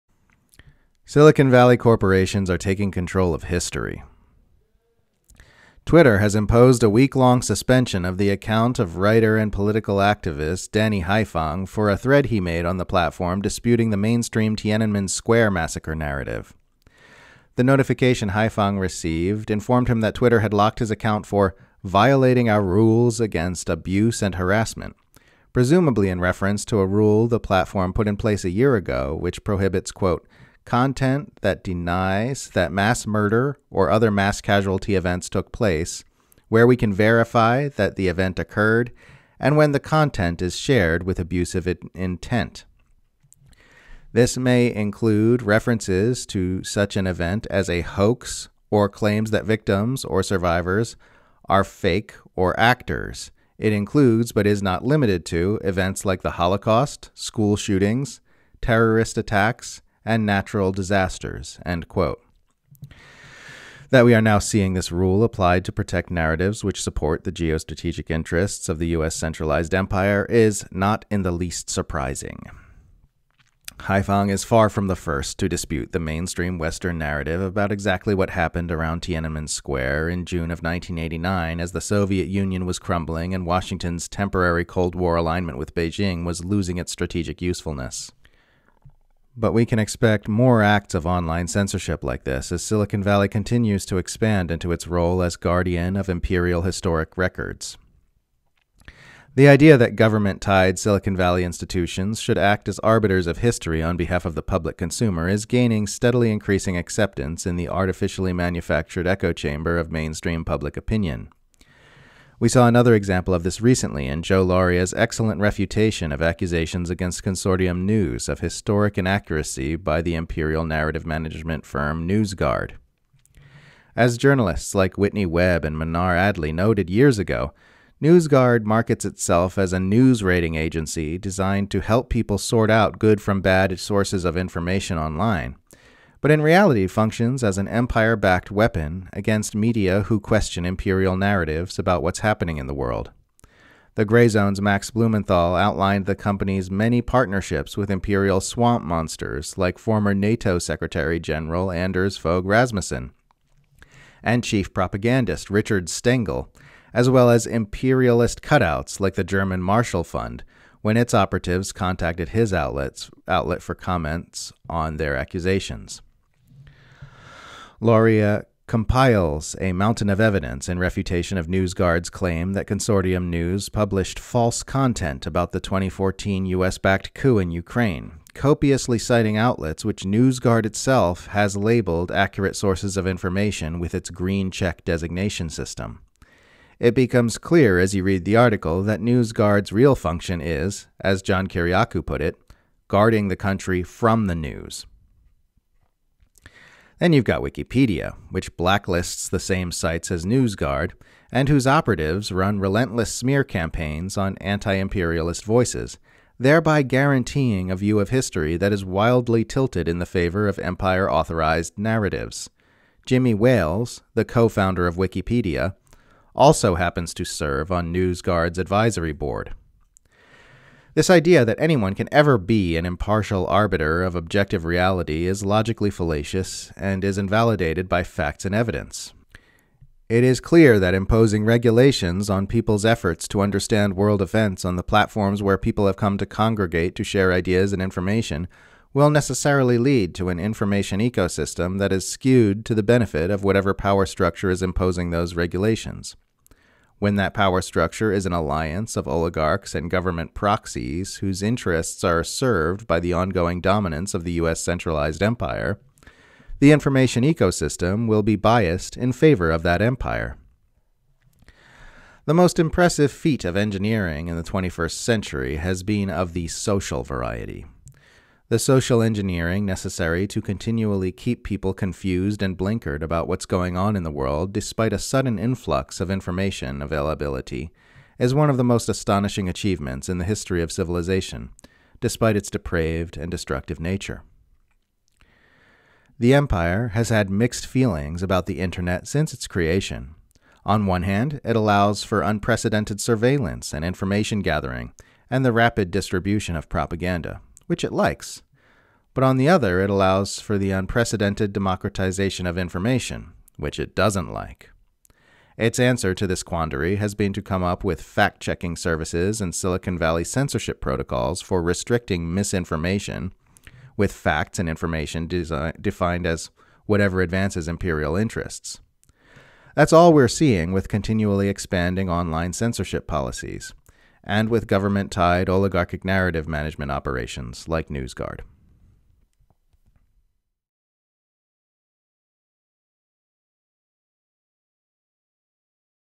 Silicon Valley Corporations Are Taking Control Of History
Twitter has imposed a weeklong suspension on the account of writer and political activist Danny Haiphong for a thread he made on the platform disputing the mainstream Tiananmen Square massacre narrative.

The notification Haiphong received informed him that Twitter had locked his account for "Violating our rules against abuse and harassment," presumably in reference to a rule the platform put in place a year ago which prohibits "content that denies that mass murder or other mass casualty events took place, where we can verify that the event occured, and when the content is shared with abusive intent."

"This may include references to such an event as a 'hoax' or claims that victims or survivors are fake or 'actors,'" Twitter said of the new rule. "It includes, but is not limited to, events like the Holocaust, school shootings, terrorist attacks, and natural disasters."

That we are now seeing this rule applied to protect narratives which support the geostrategic interests of the US-centralized empire is not in the least bit surprising.